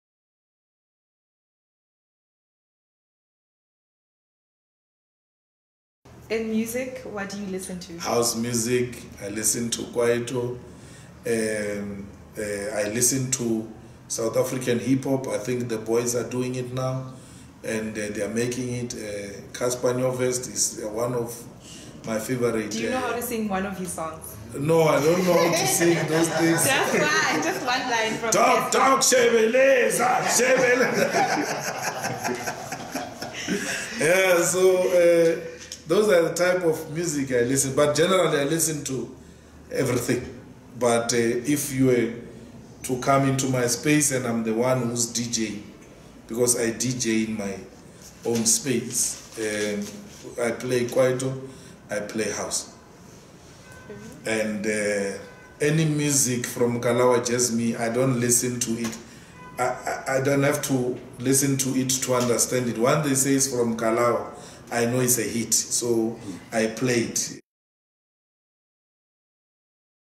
Malema listens to Cassper Nyovest and Kalawa hits
Ntombi Mlangeni interviews Economic Freedom Fighter (EFF) CIC, Julius Malema and discovers his local taste in music. He listens to @CassperNyovestOfficial and hits from @Kalawa Jazmee TV